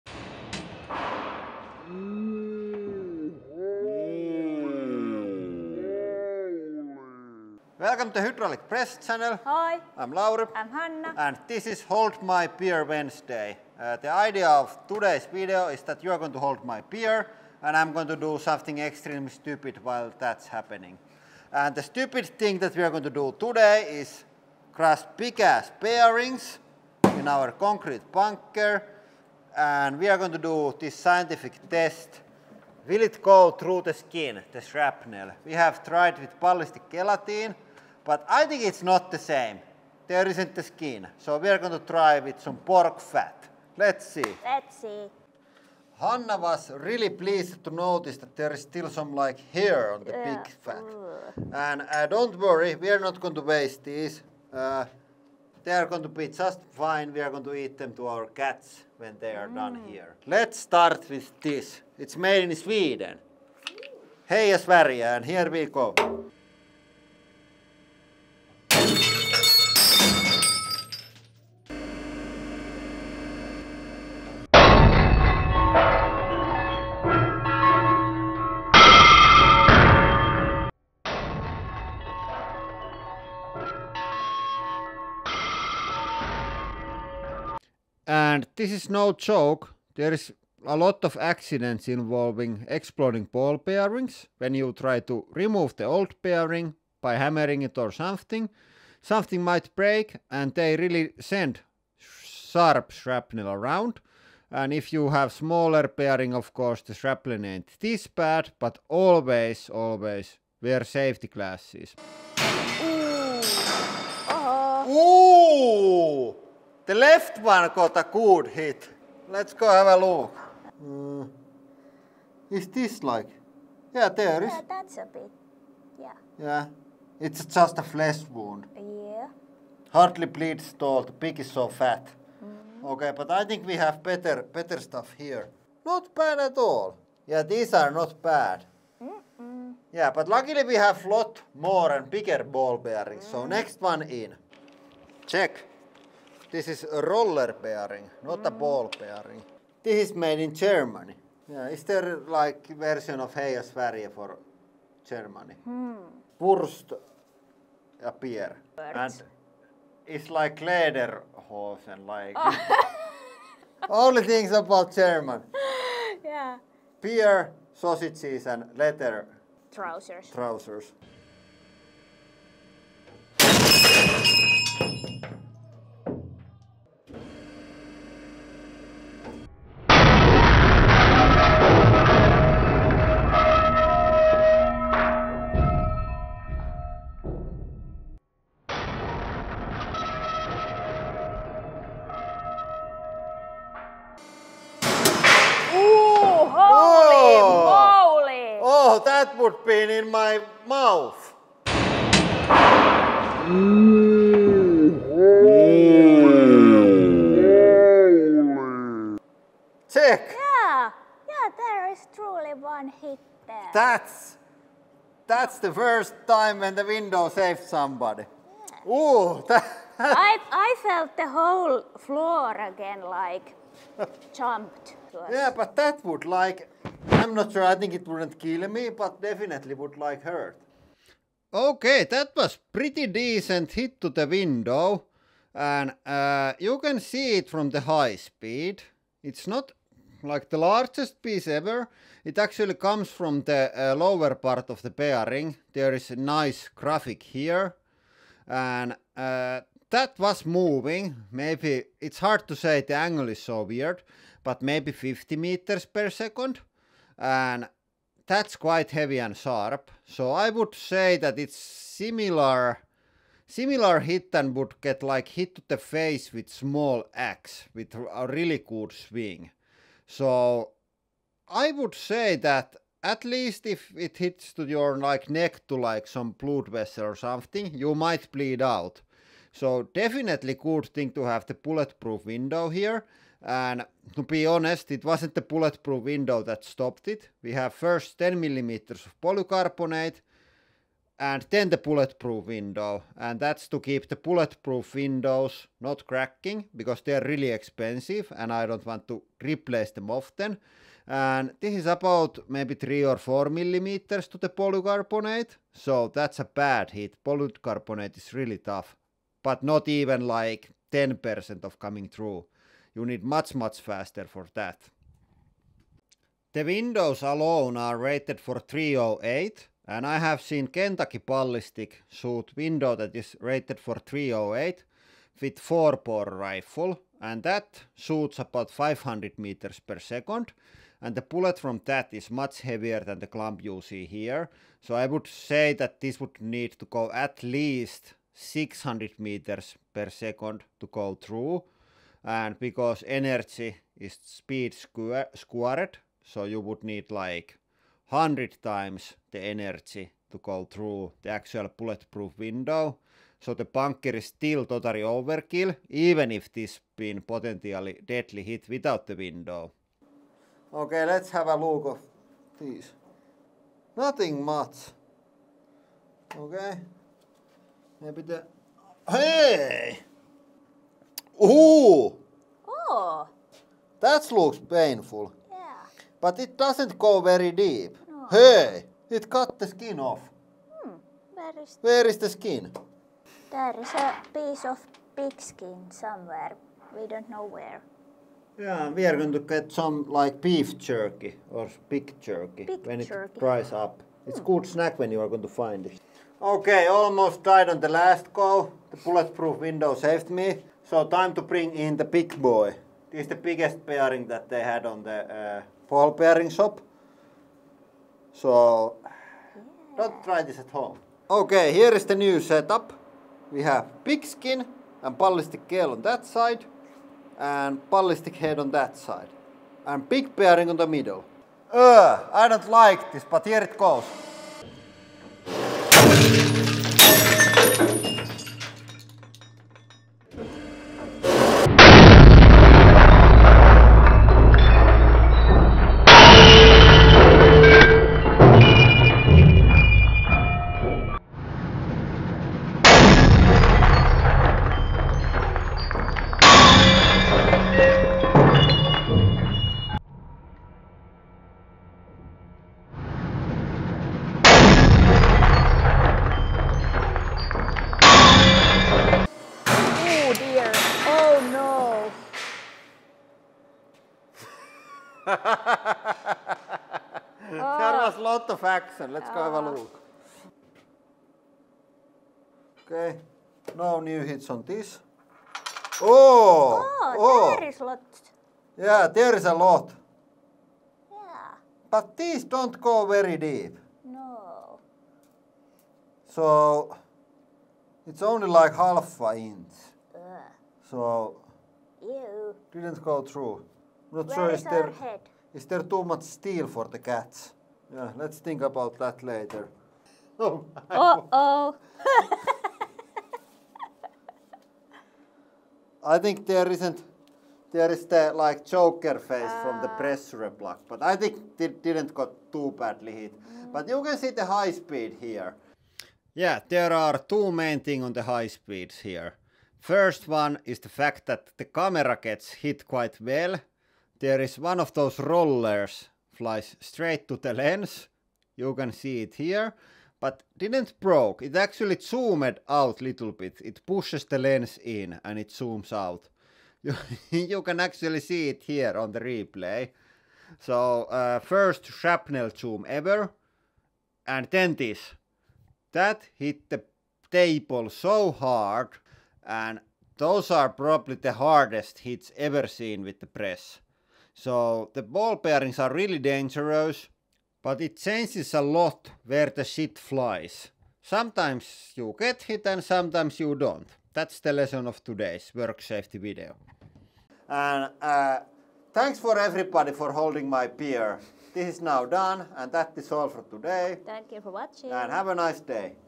Saved from Deadly Ball Bearing Shrapnel – Bulletproof Window Did Its Job!
We decide to test how well ball bearing shrapnel pierces skin in the name of work safety research but we ended up testing instead how tough are our bullet proof windows! The windows have 10mm polycarbonate wear plate and then 50mm/ 2 inch bullet proof glass that is rated for .308 rifle rounds so the ball bearing shrapnel was easy task for it but still really exiting day at the office!